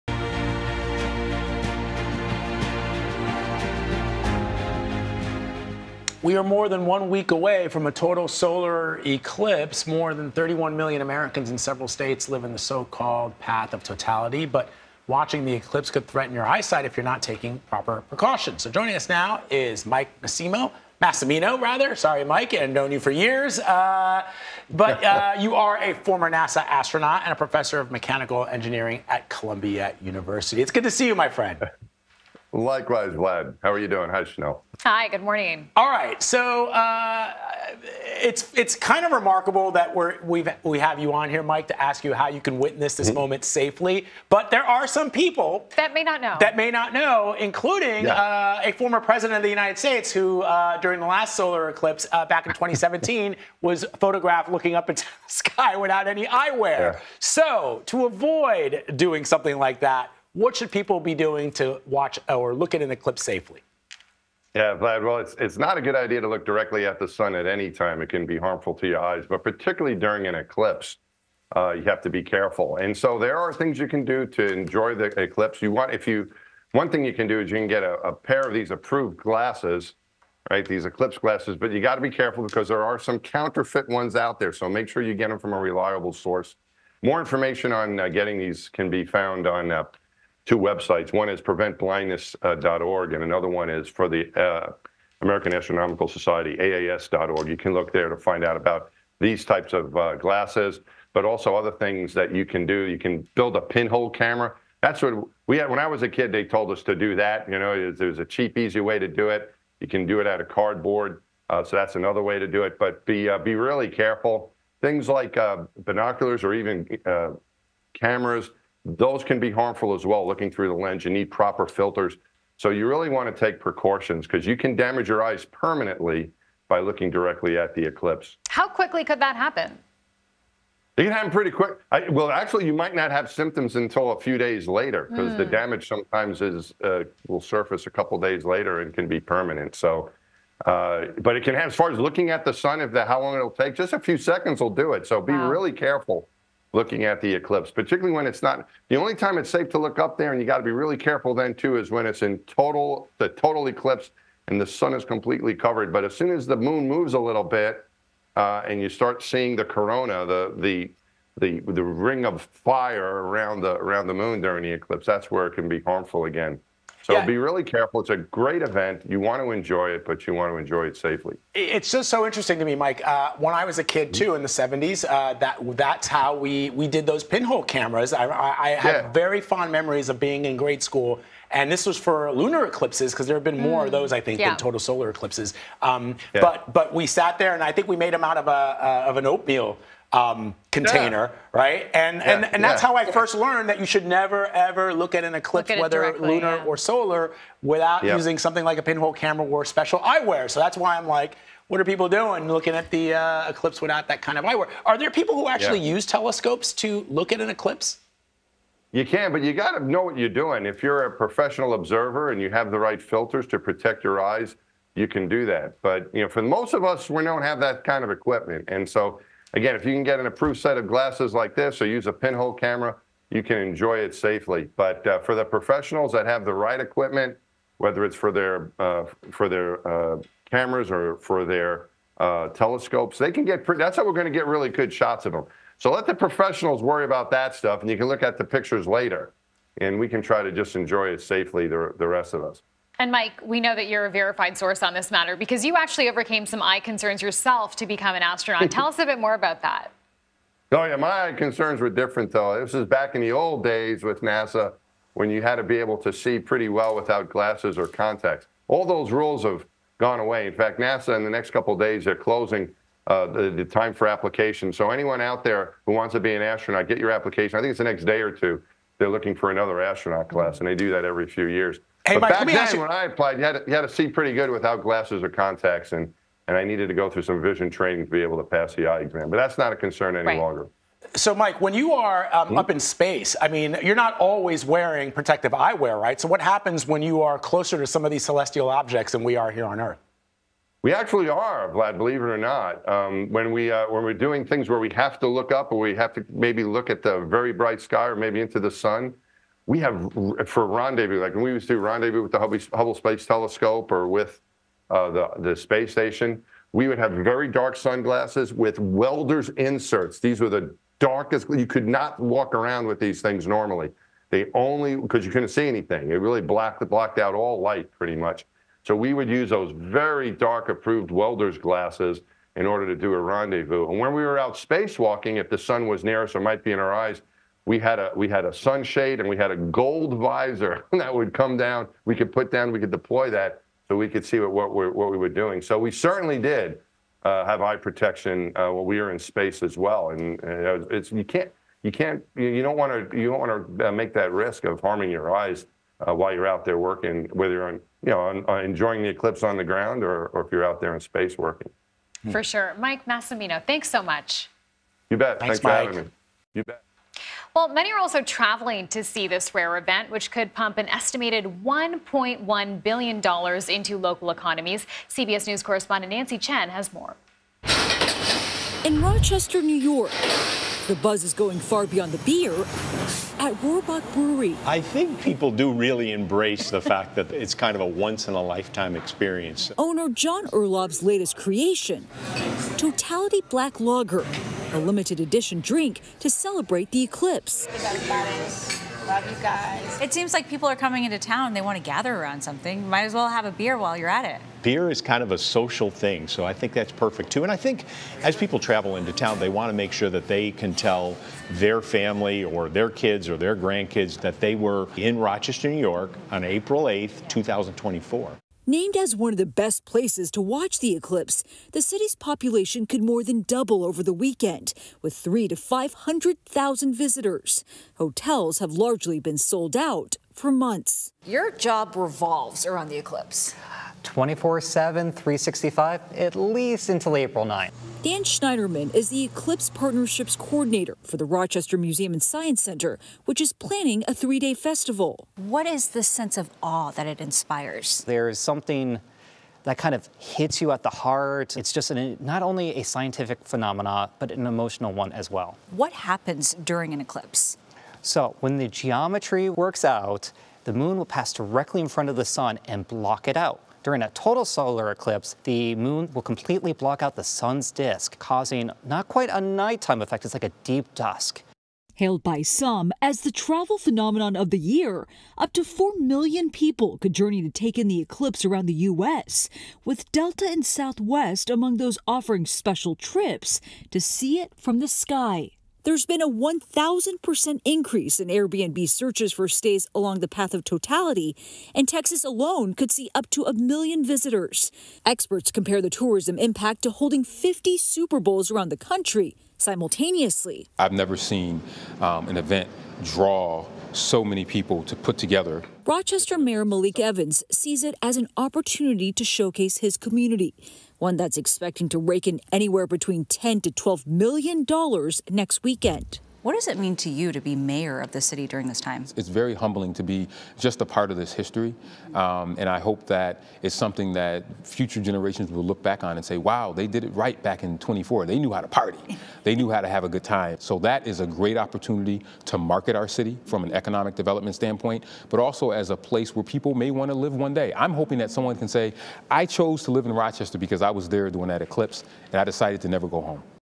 How to keep your eyes safe during the solar eclipse
More than 31 million Americans live in the path of next week's solar eclipse, but watching it could threaten your eyesight if you're not taking proper precautions. Former NASA astronaut Mike Massimino, a professor of mechanical engineering at Columbia University, joined CBS News with safety tips before watching the eclipse. And CBS News correspondent Nancy Chen reports on the millions expected to travel for a look at the solar event.